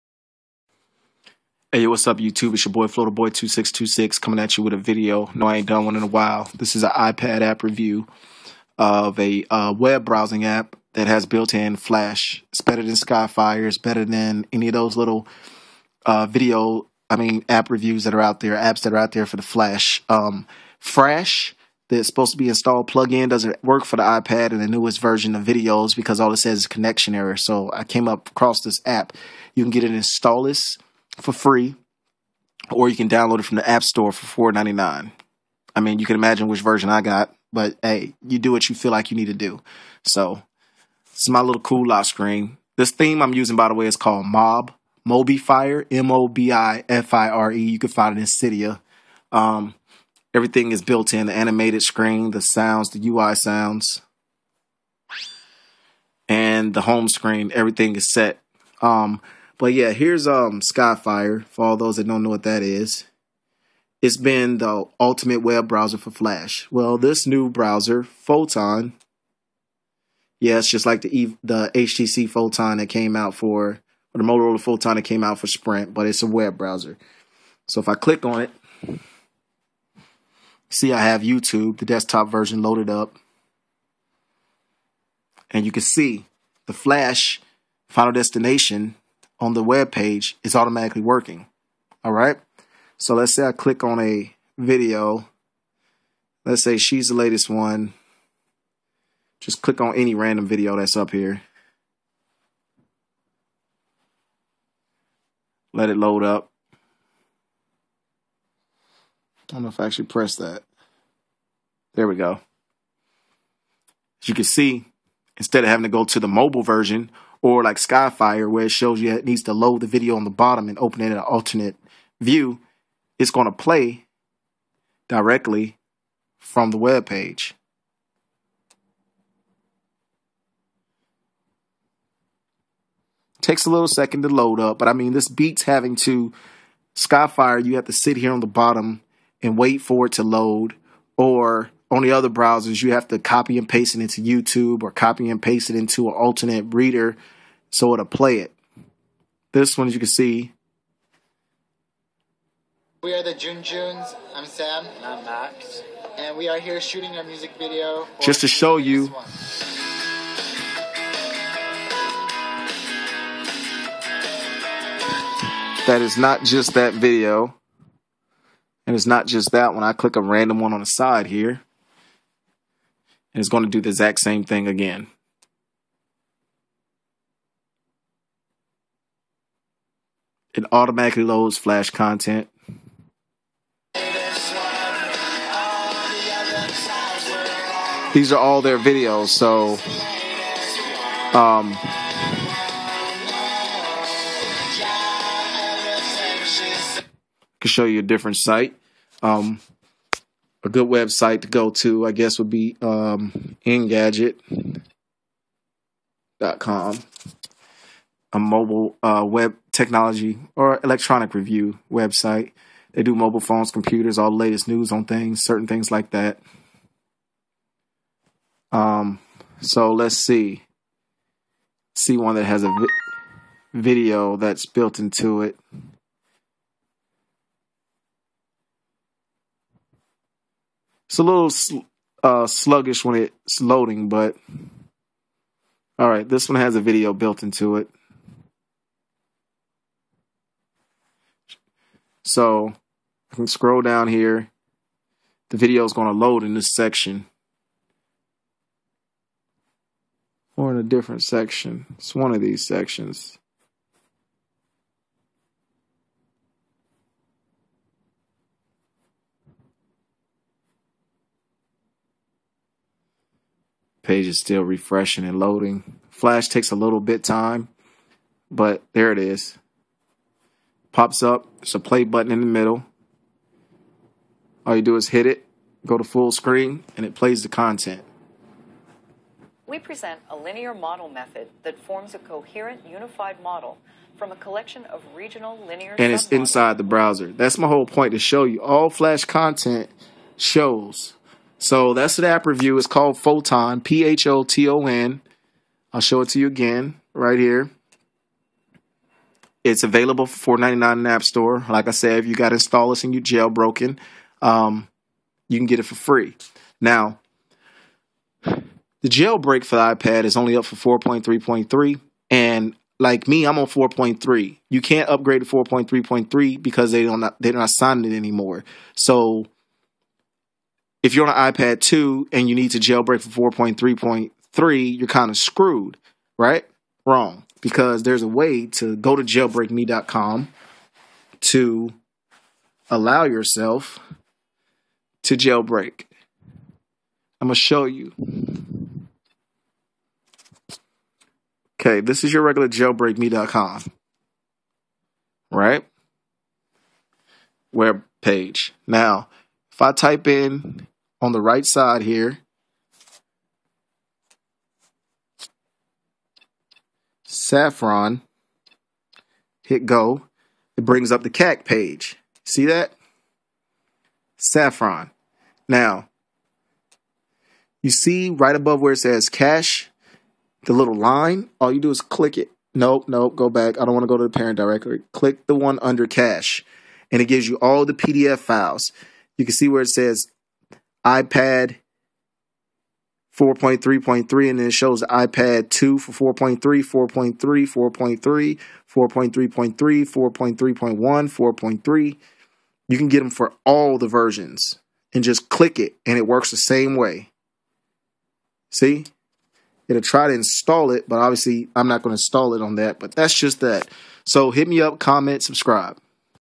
Flash for the ipad best app out
This app is called photon available @n the appstore for $4.99 or in installous.